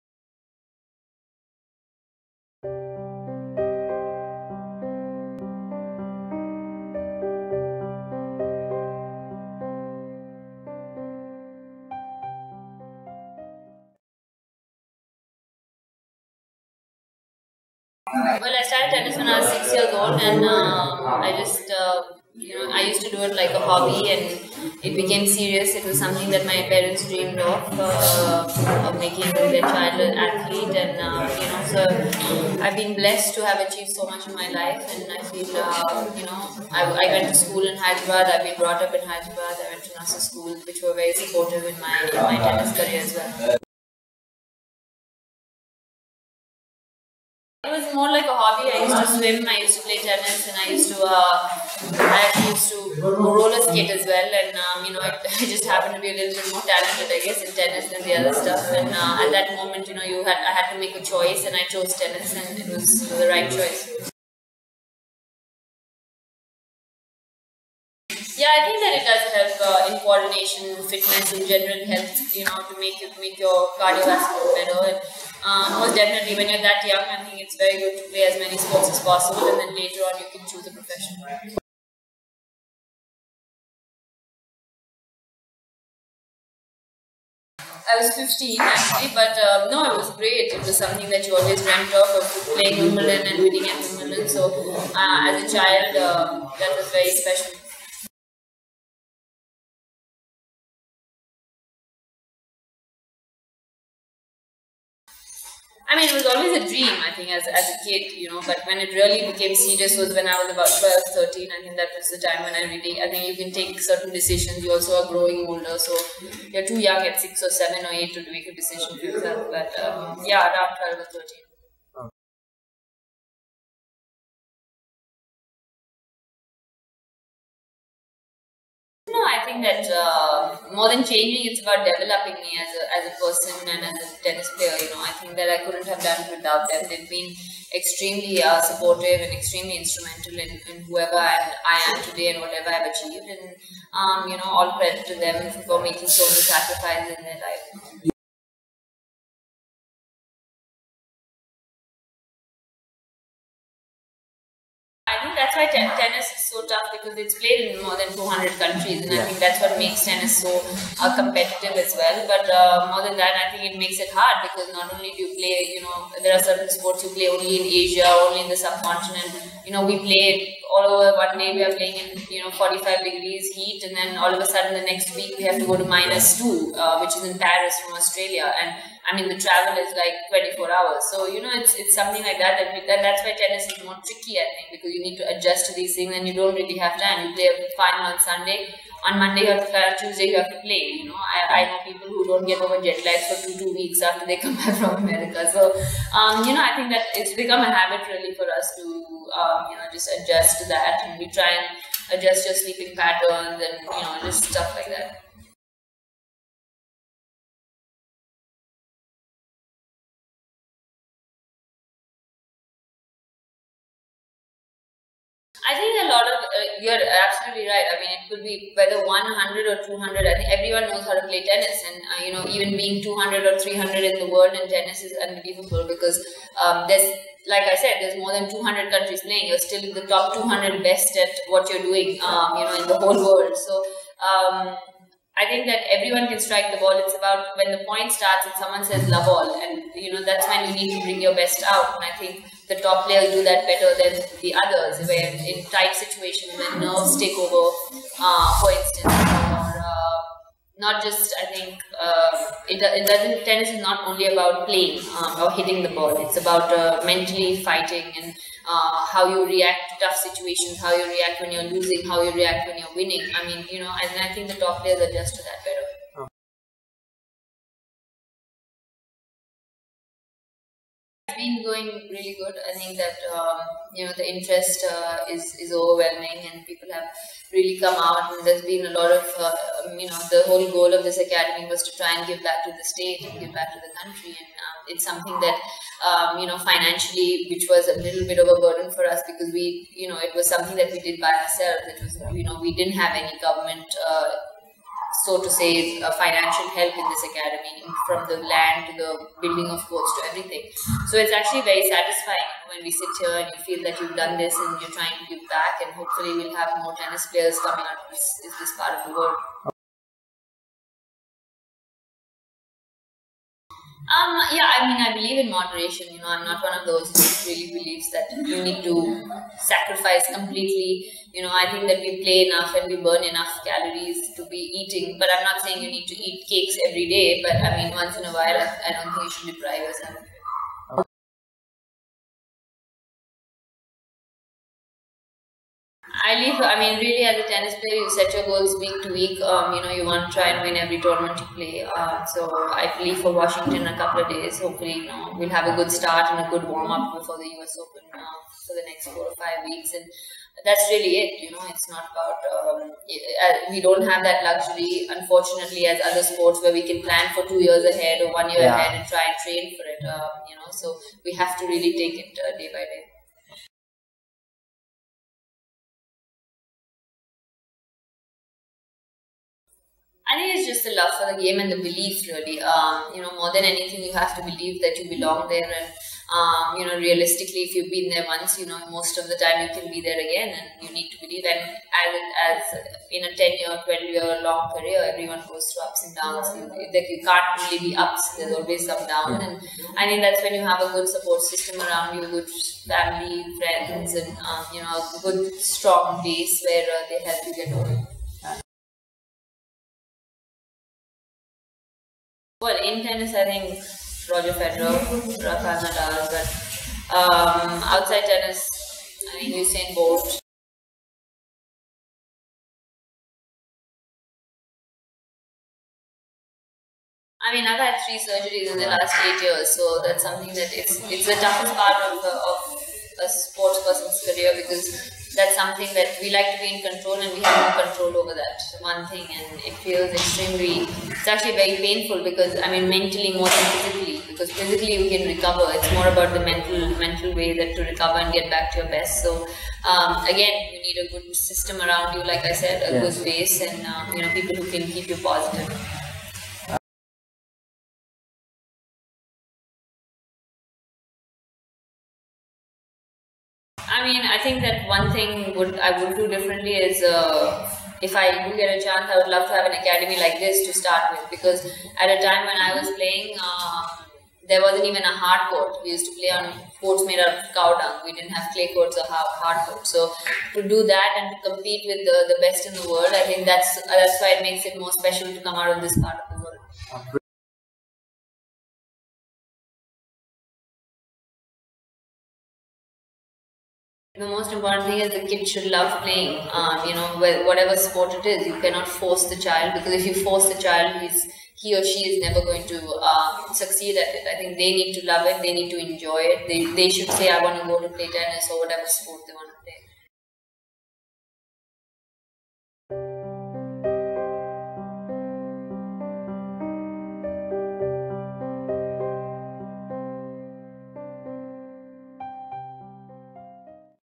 Inspirational Stories : Sania Mirza shares her experiences with DAS
Congratulations on winning US Open Mixed doubles title from dream about sports community ..Way to go!!!

You have showed following a dream with Passion, determination and hard work ,everything is possible.

to know more!